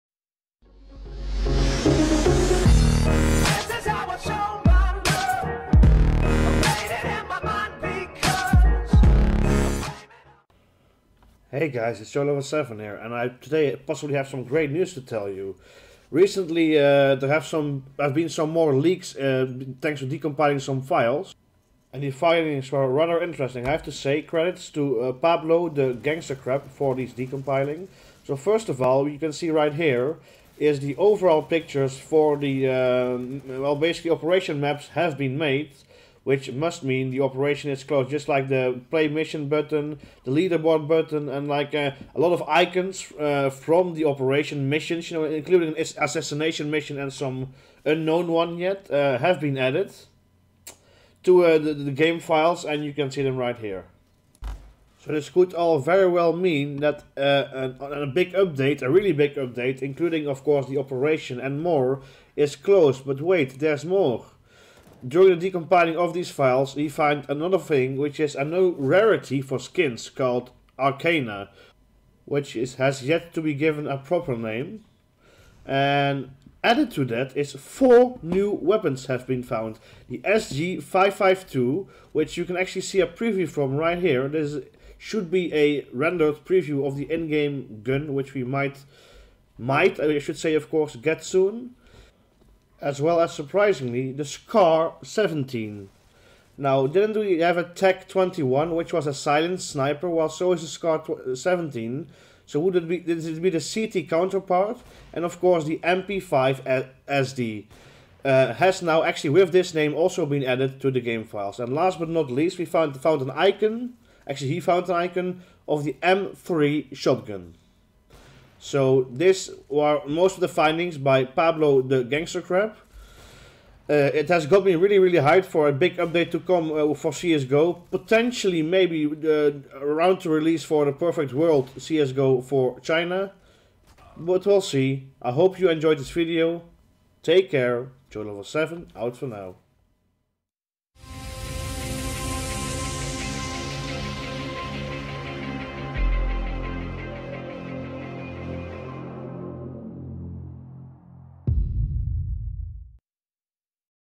CS:GO NEW LEAKED UPDATE INFO (New Content & Operation)
Credits for the findings to Pablo the Gangster Crab.

Icons for 4 new guns, Assassination game mode, Guardian and Co-Op Strike game mode, one new strange game mode, references to Co-Op missions and other missions AND some strange other icons, new skin rarity and more.

counter-strike global offensive,update news,operation,leaked,info